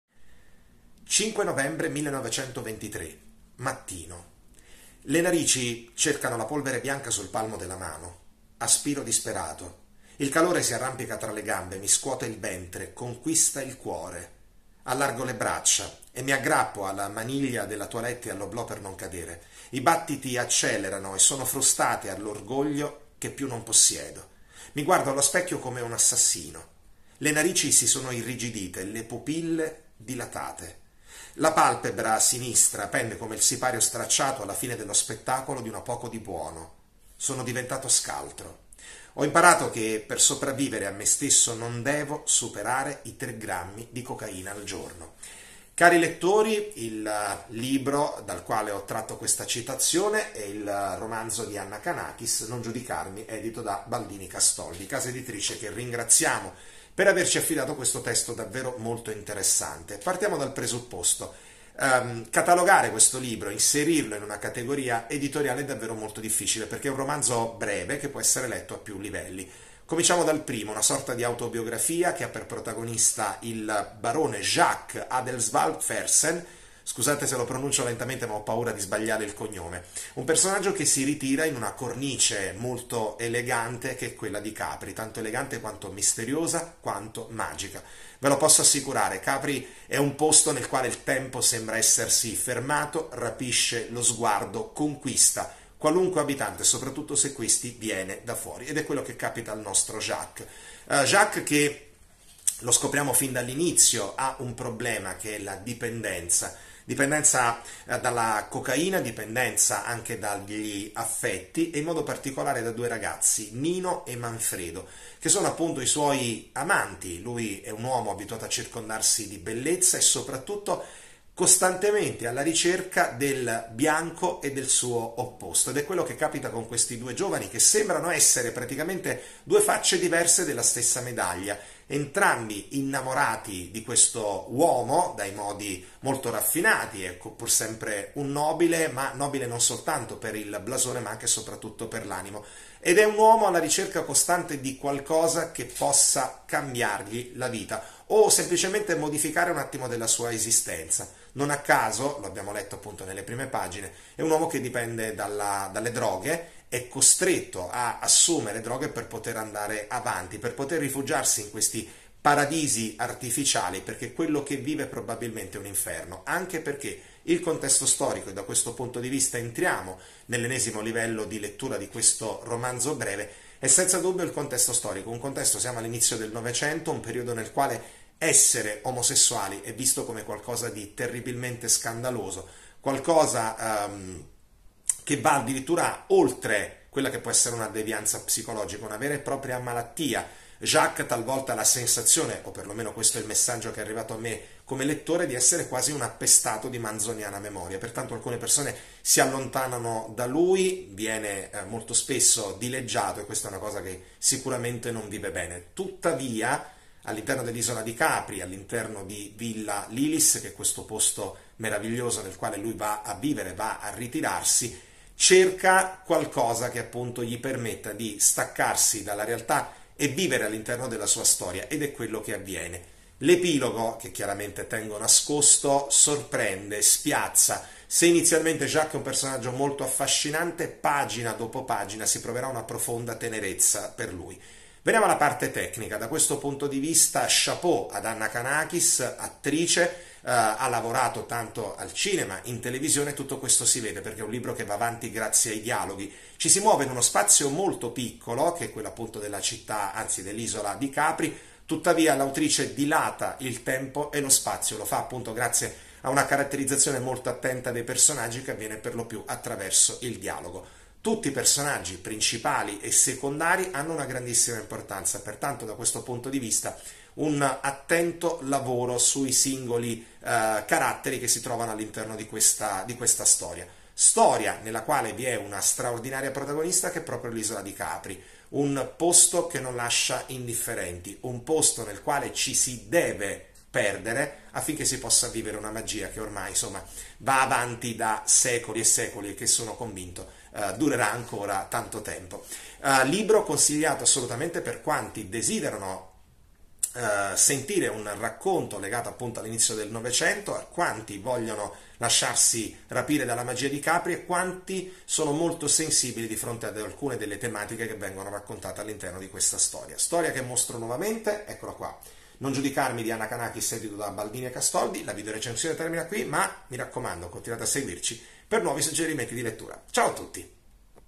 La videorecensione del Lettore Medio - Non giudicarmi
Paquito recensisce il nuovo libro di Anna Kanakis edito da Baldini+Castoldi